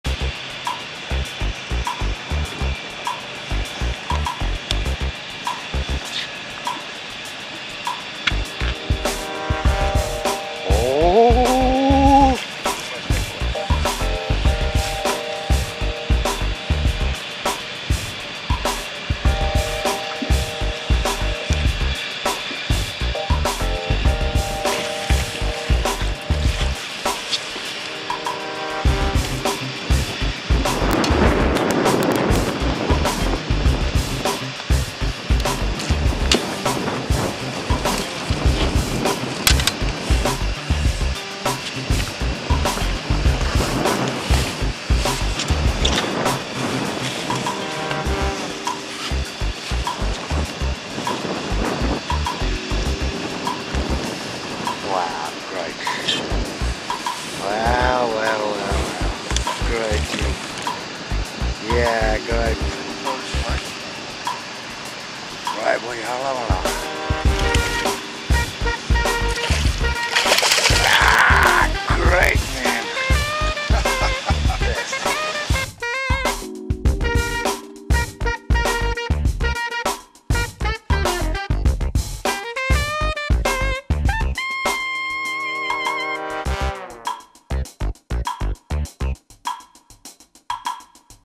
Fishing Guide Amsterdam
Zander fishing in the Netherlands with Pro-staff angler Michael Stalenhoef.(dezaansevisgids.nl)